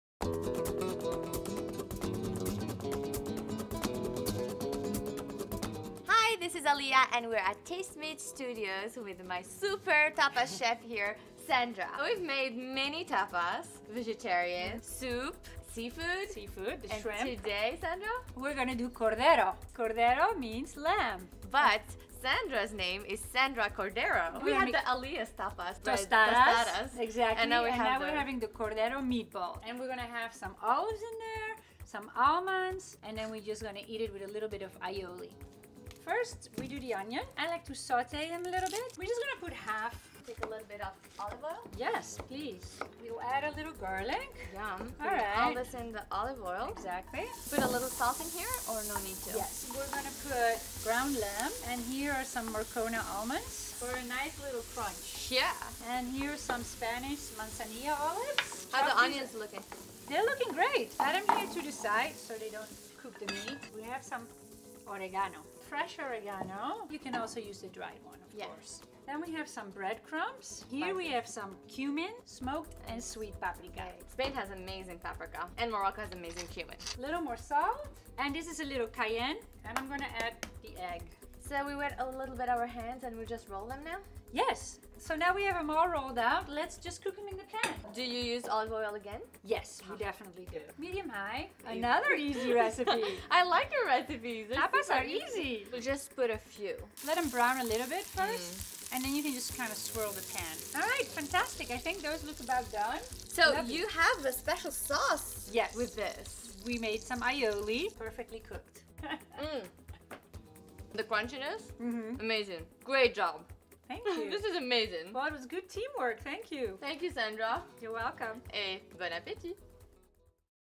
Lamb Meatballs Recipe - Summer Tapas Series, Tapas 4 - CookingWithAlia - Episode 265
It's summer time. It's tapas time! Check out this special Summer Tapas Series with my friend Chef Sandra Cordero from The Cordero Negro Fourth Tapas in our series of 5 Tapas are delicious lamb meatballs!

INGREDIENTS:
1 pound (450 grams) ground lamb
1 egg
1/3 cup bread crumbs
16 spanish olives (Manzanilla) chopped
2 tablespoons Marcona almonds (or roasted almonds) chopped
1 tablespoon fresh oregano or mint (chopped)
1 cup yellow onion (diced)
1 tablespoon chopped garlic
3 tablespoons olive oil
1 teaspoon sweet paprika
1 teaspoon smoked paprika (optional - replace with sweet paprika)
1/2 teaspoon ground cumin
A pinch of cayenne pepper
1 teaspoon salt, and 1/2 teaspoon black pepper